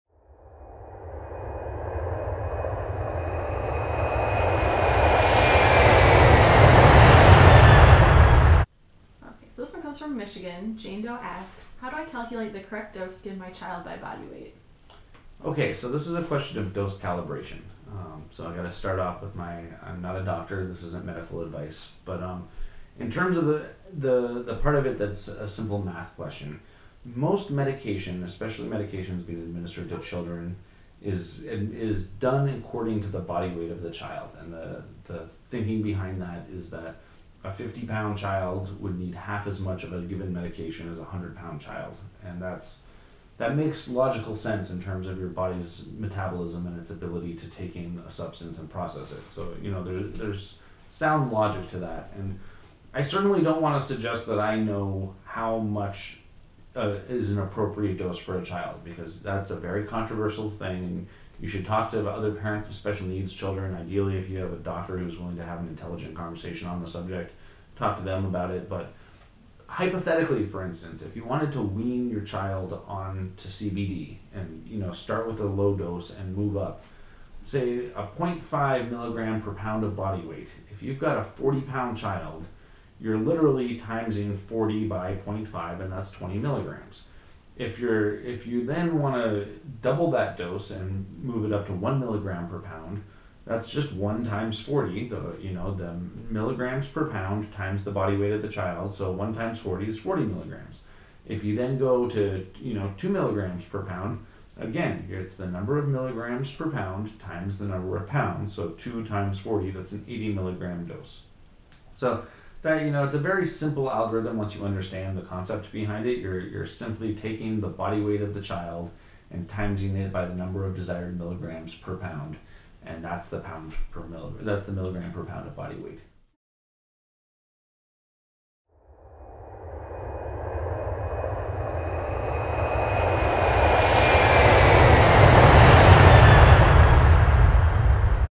Cannabis CBD Dose Calibration by Body Weight.
How to calculate the correct dose of CBD by body weight.  Pro Plant Ware and The Cannabinoid Chronicles are working to raise awareness about the science of medical cannabis.  Our goal is to encourage the world to Rethink Cannabis. for more information.